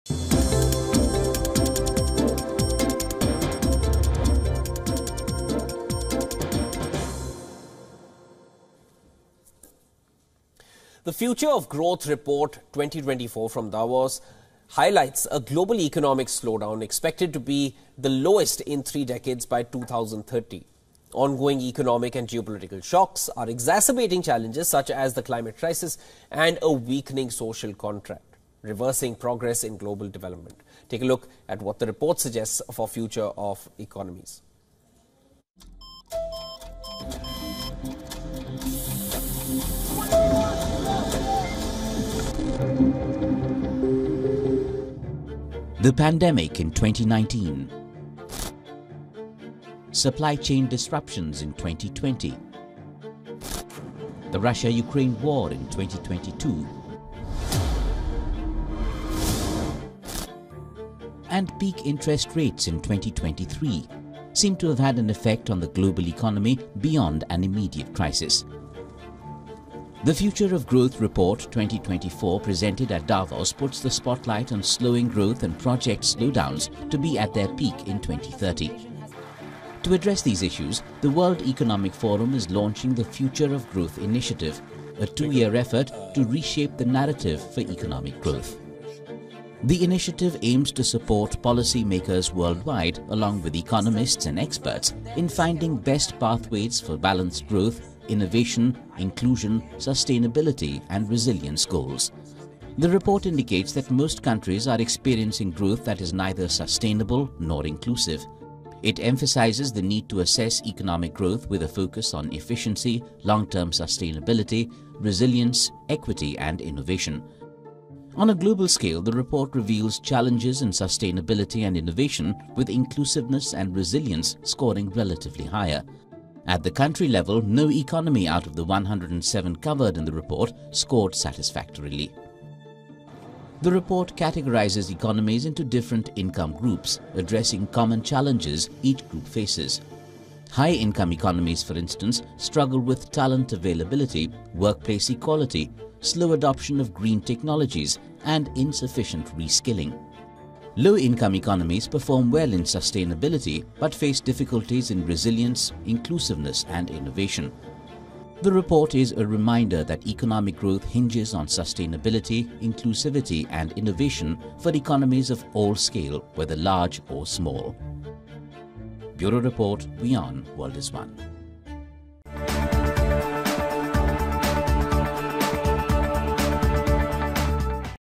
2030 expected to be year of slowest growth in 3 decades | World Business Watch
The future of growth report 2024 from Davos highlights a global economic slowdown expected to be the lowest in three decades by 2030. ongoing economic and geopolitical shocks are exacerbating challenges such as the climate crisis and a weakening social contract, reversing progress in global development.